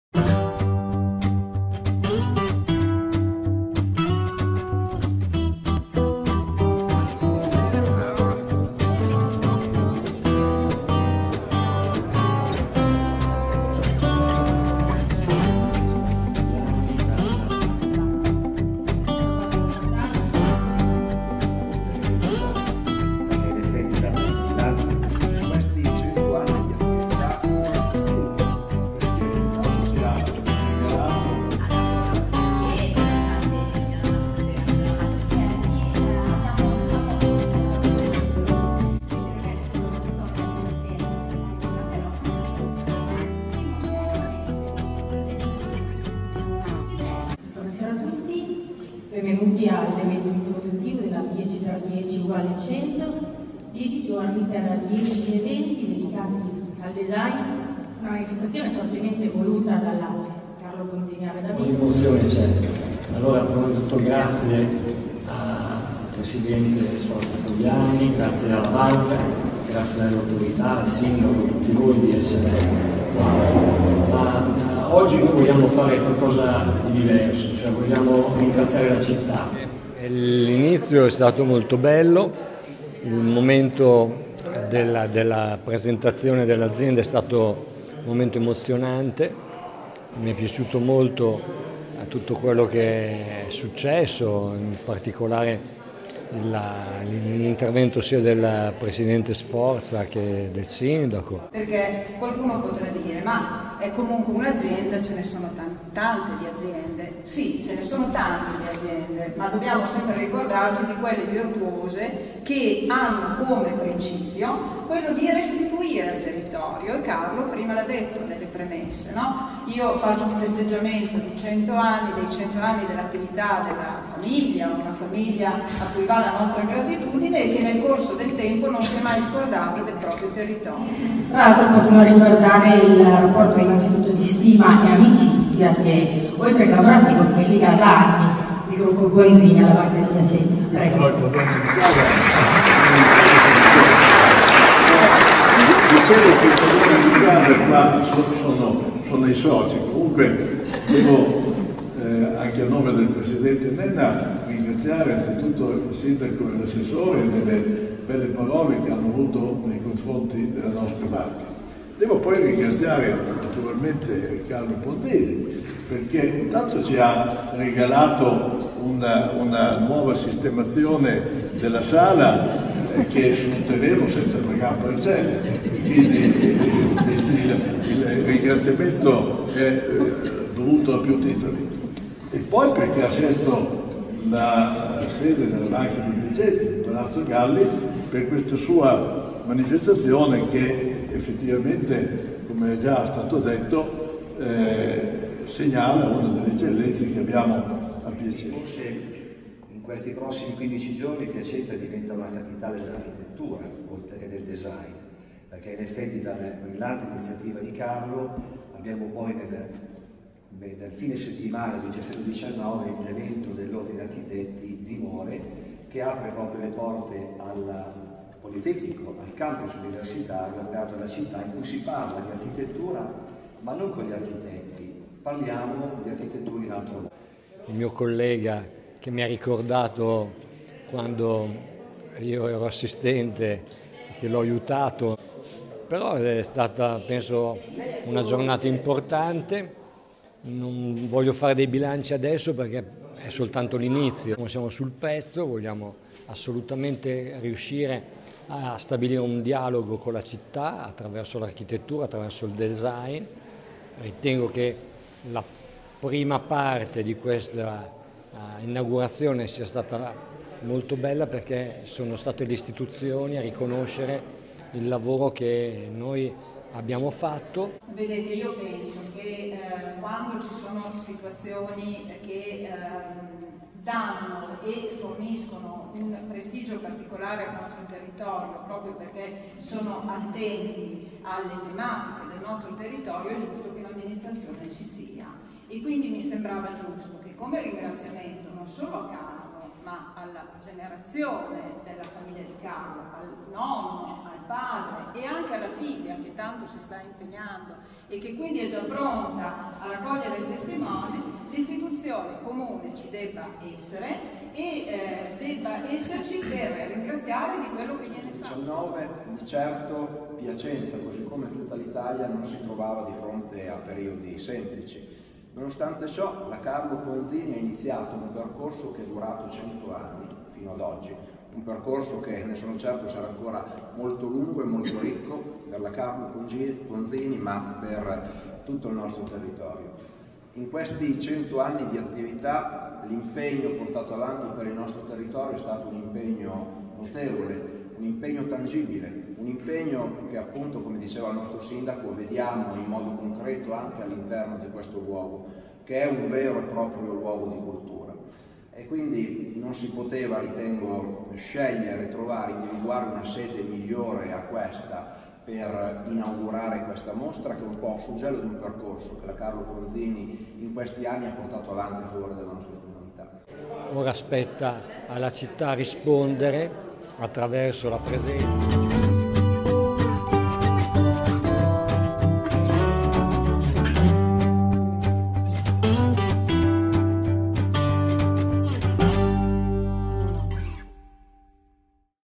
"Ponzini Arredamento 100 anni" inaugurazione dell'evento
Le immagini e le interviste della inaugurazione del festival del design proposto dall'azienda piacentina in occasione dei 100 anni di attività